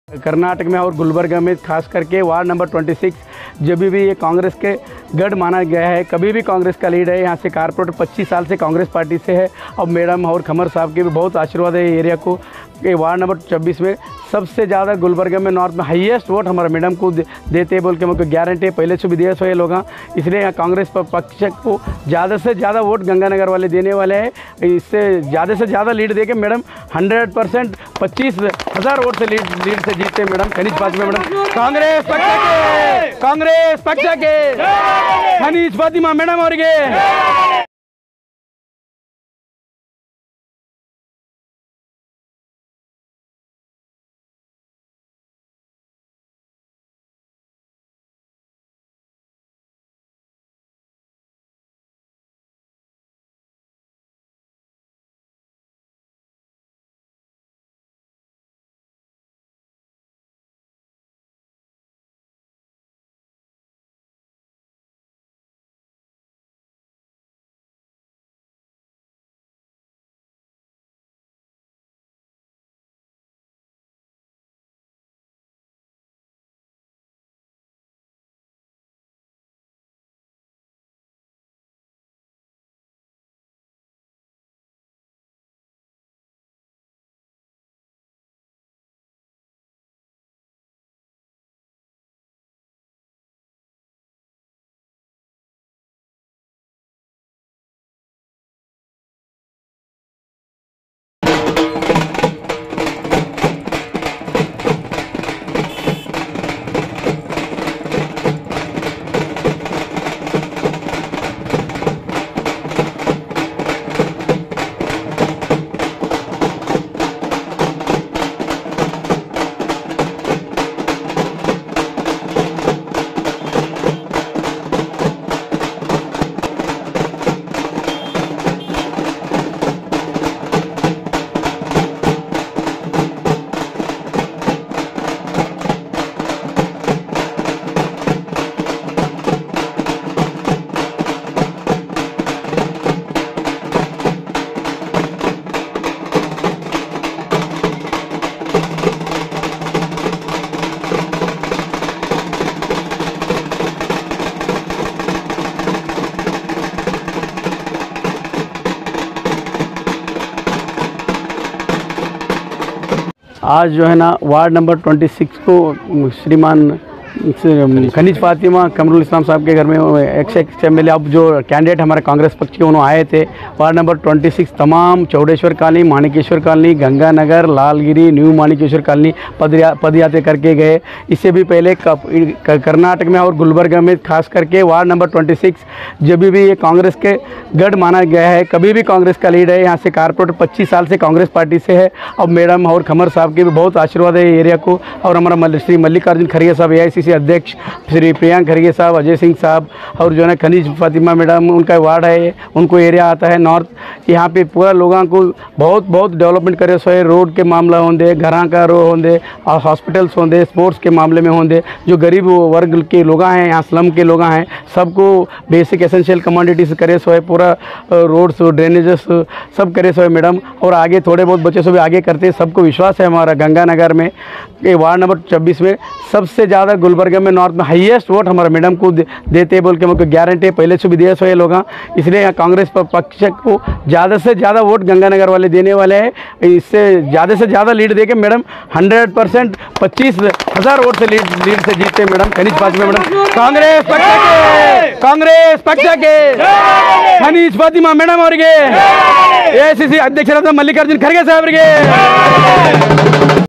Ward No 26 Door To Door Campaign Under The Leadership of Gulbarga North Congress Party Candidate Mad
Ward No 26 Door To Door Campaign Under The Leadership of Gulbarga North Congress Party Candidate Madam Kaneez fatima Saheba